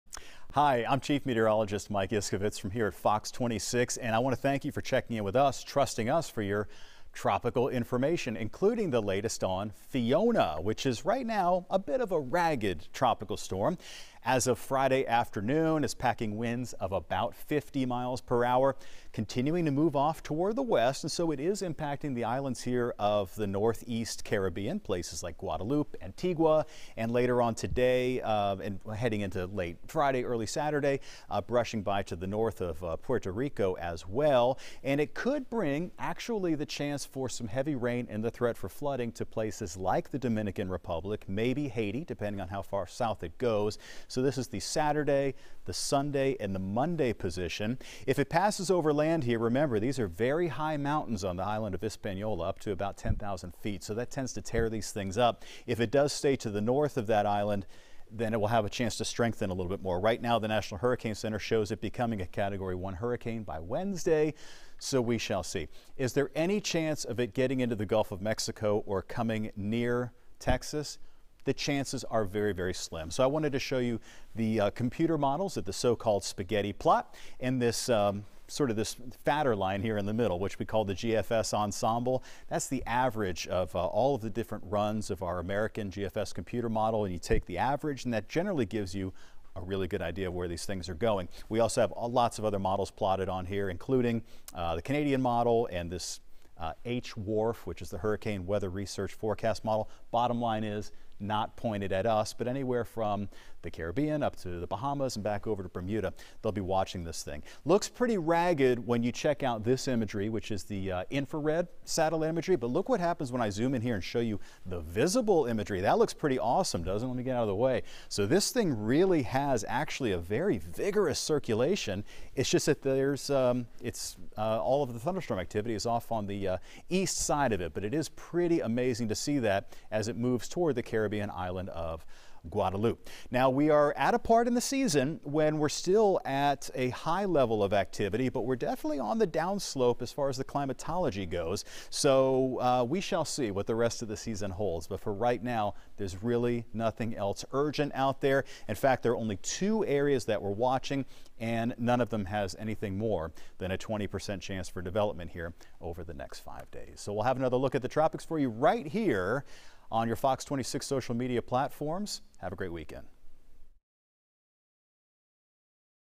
Tropical Weather Forecast - September 16, 2022
Tropical Storm Fiona remains weak, but looks amazing on visible satellite! Will it get anywhere near Texas?